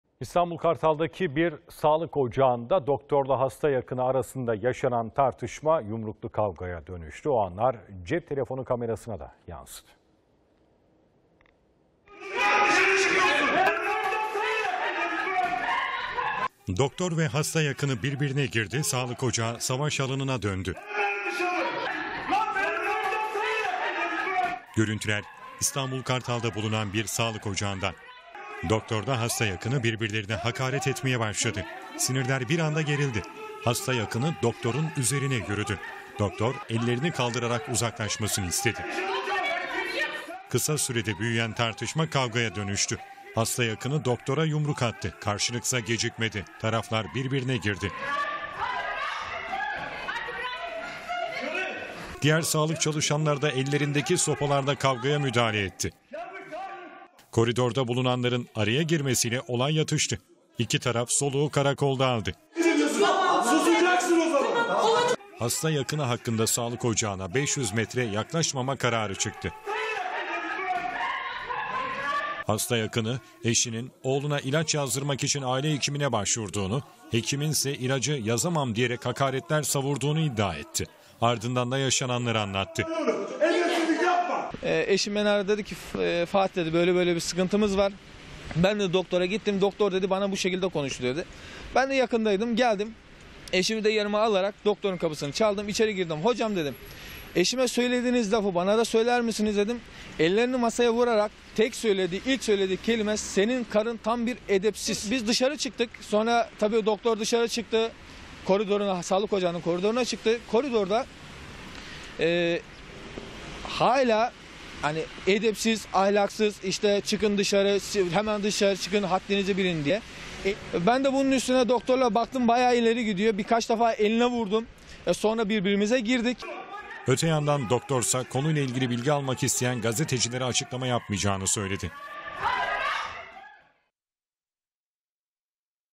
Doktor ve Hasta Yakını Birbirlerine Girdi / A Haber
Kartal’da bir sağlık ocağında doktor ile hasta yakını arasında yaşanan tartışma, yumruklu kavgaya dönüştü. O anlar cep telefonu kamerasına yansıdı. Olay sonrası hasta yakını yaptığı açıklamada, "Bildiğim kaç kişi doktorun konuşmaları, tavırları yüzünden hekimini değiştirdi” ifadelerini kullanırken doktor ise açıklama yapmadı..